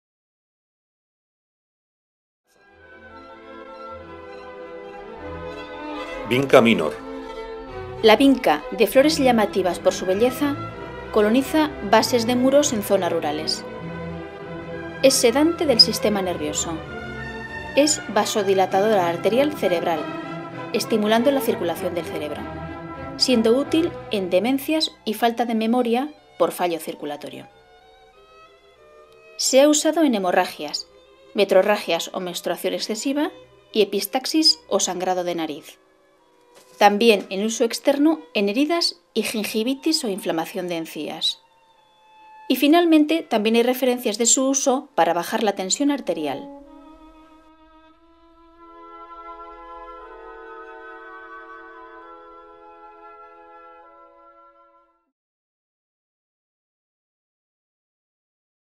Las mejores plantas para tu salud por Antonio Vega. Propiedades de la Vinca
Vinca mejora la circulación del cerebro.